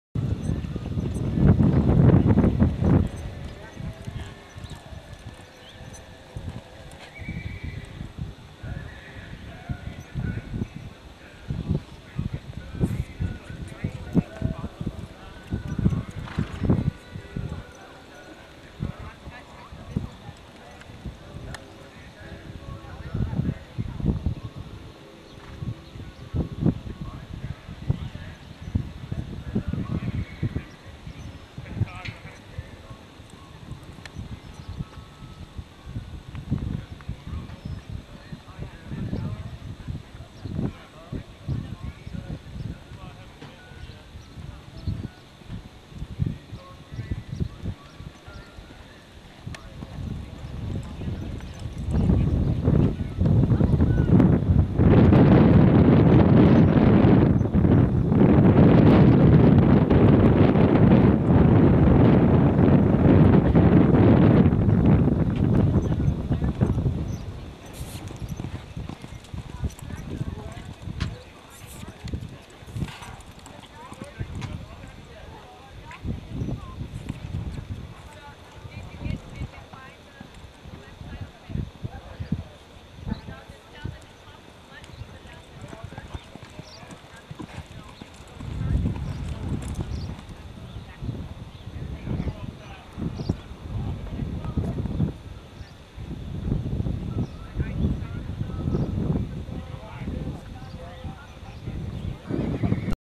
Rooster (kickers korner)
rooster working the barrels, then cruizing through the pattern